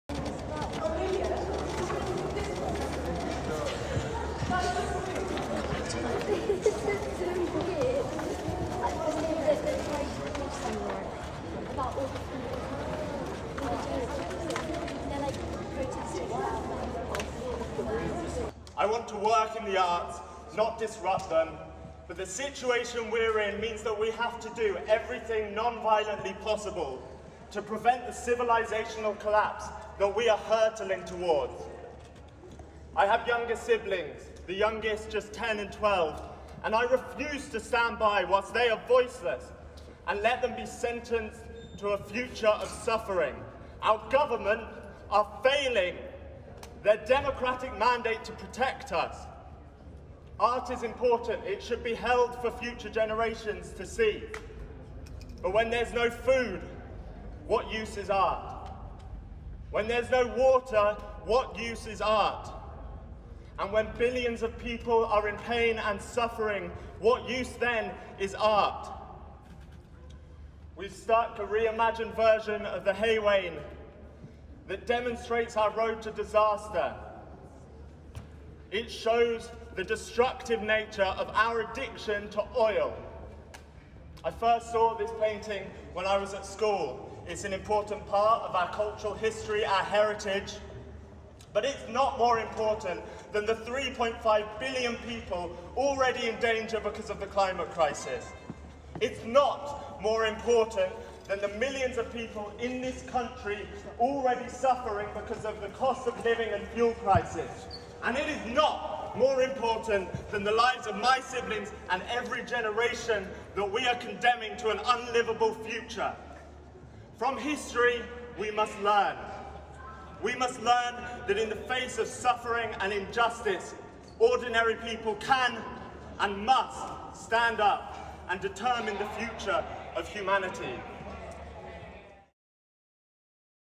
Just Stop Oil protestors glue themselves to John Constable masterpiece in National Gallery
Two Just Stop Oil eco-warriors have now glued themselves to the frame of a Constable painting the Hay Wain at the National Gallery.

The protesters, who said they were a couple, shouted their demands for the government to end new extractions for oil and gas as their hands were glued to the masterpiece described as one of the “greatest and most popular English paintings” ever.

Student activist Hannah Hunt, 23, shouted to onlookers: “Yes my hand is glued to this painting but there is blood on the hands of the government.”

A National Gallery Spokesperson said police have been called to the exhibit which has been closed to the public.